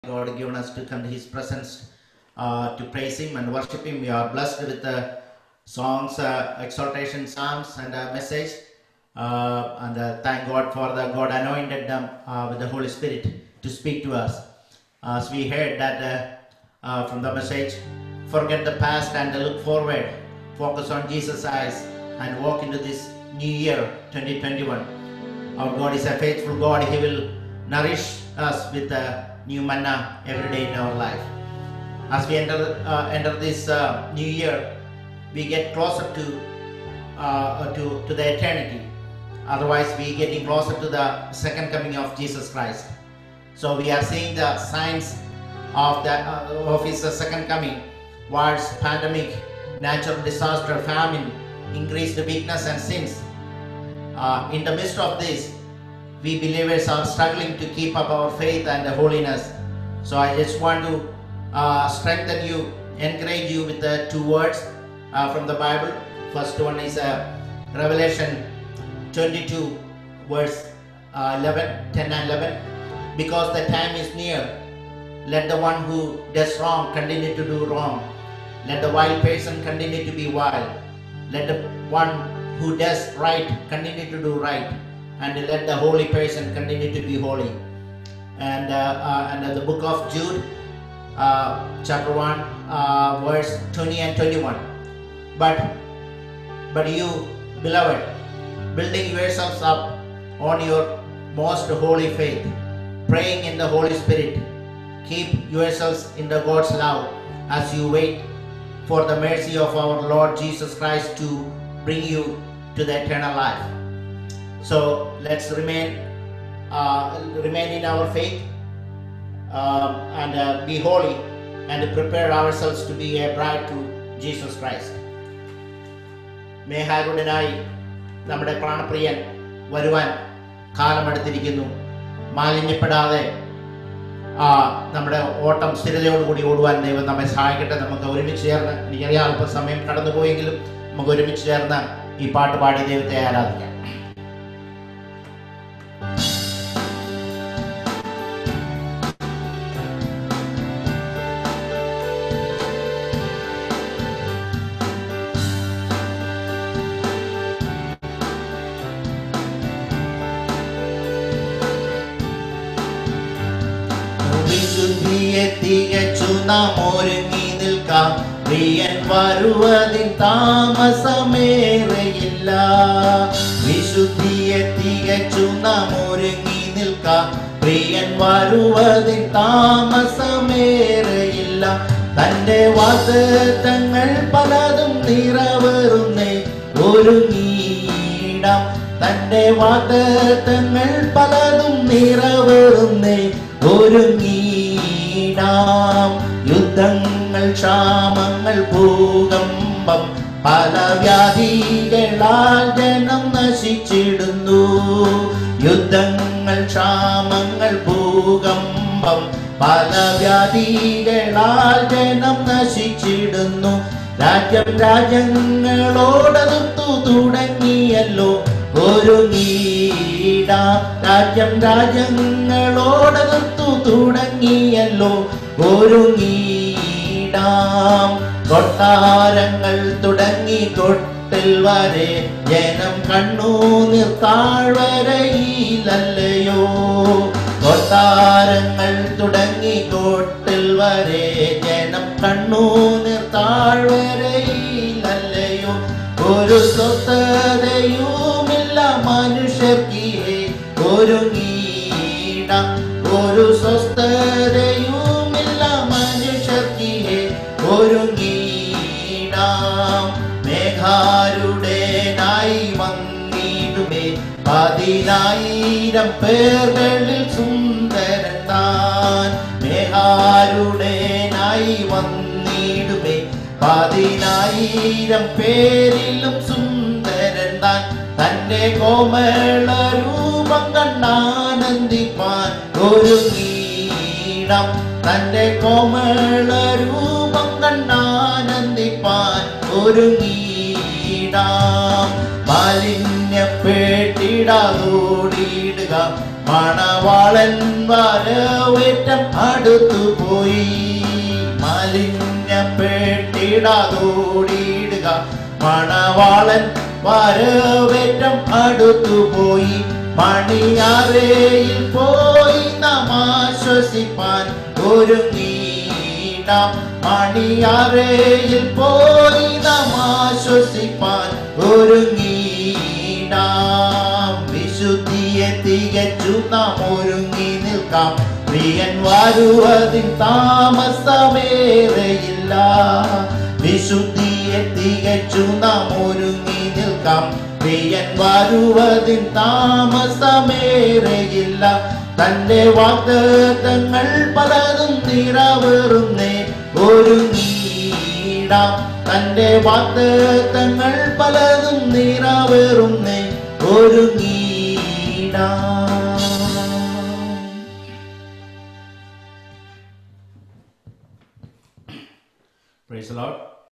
ELCoG 2021 1 3 Sunday Worship Part 2
Eternal Life Church of God
Sunday Worship - January 3rd, 2021

Worship Team:
Vocals: Josen George, Joselyn George
Keyboard: Justin George